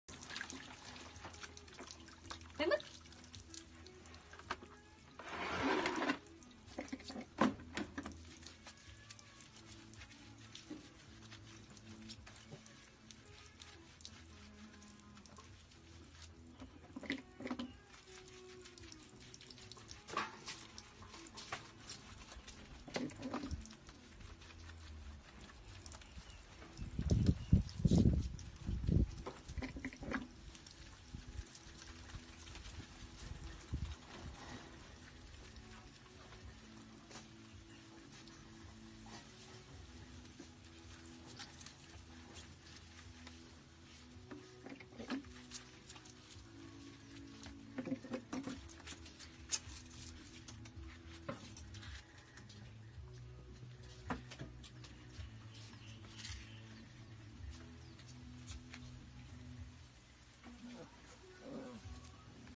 This is the way we take a bath lovely dog
Taking a bath our lovely dog named Carlo